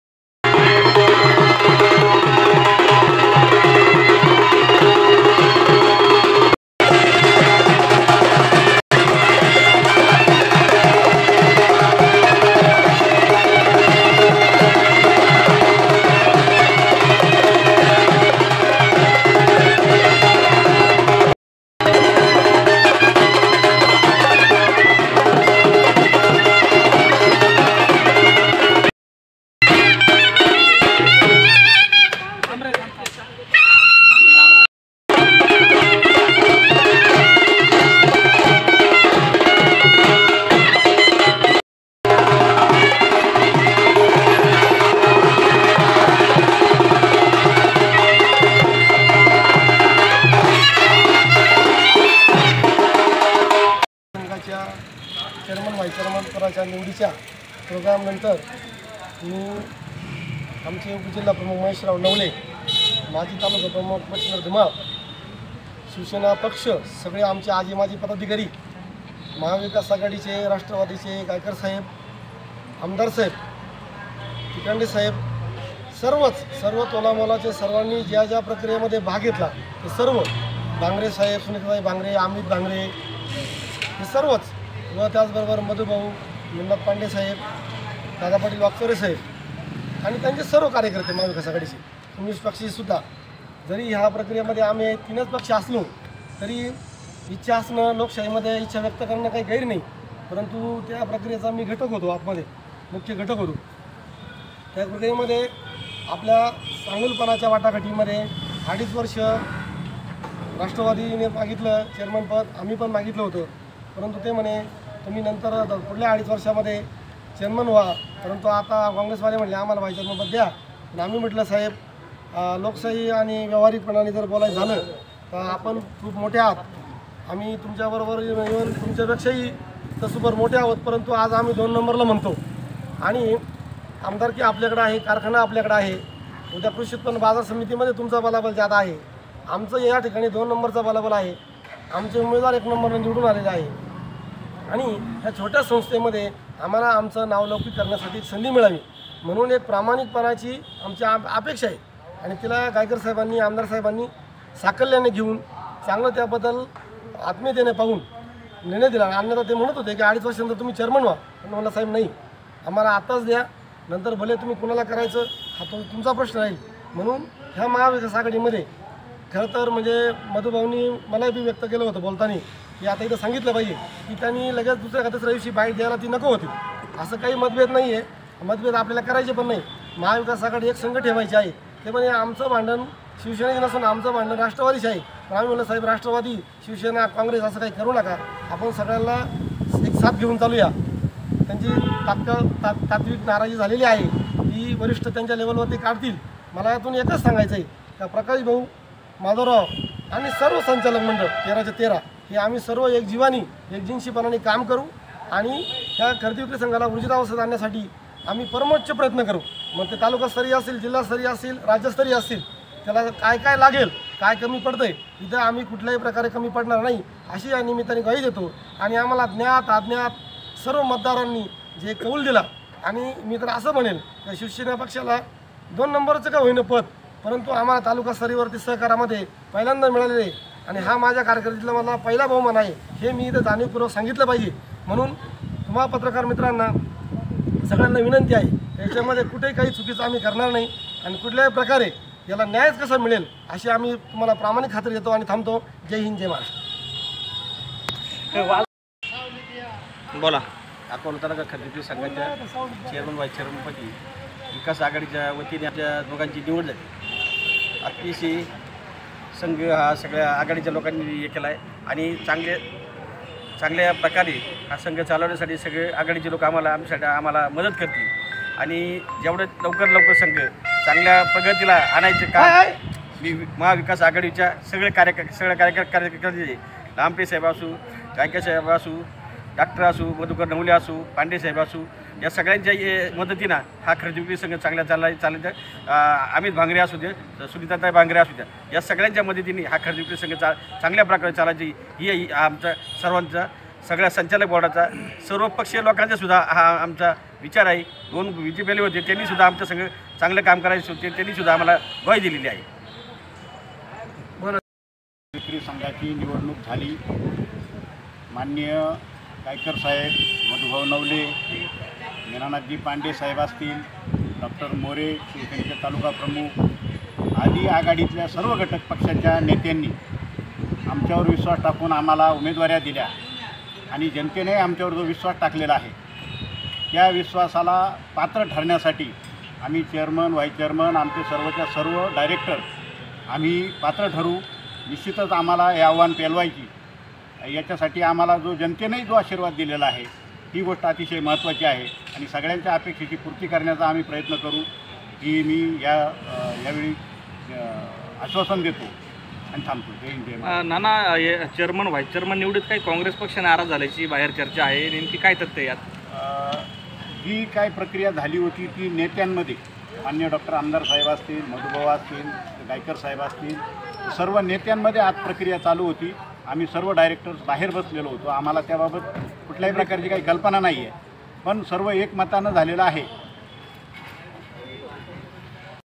अकोले तालुका खरेदी विक्री संघाच्या चेअरमन पदी नाईकवाडी तर व्हा.चेअरमन पदी तिटमे यांची निवड
अकोले तालुका खरेदी विक्री संघाच्या चेअरमन पदी प्रकाश नाईकवाडी तर व्हा.चेअरमन पदी माधवराव तिटमे यांची निवड | अकोले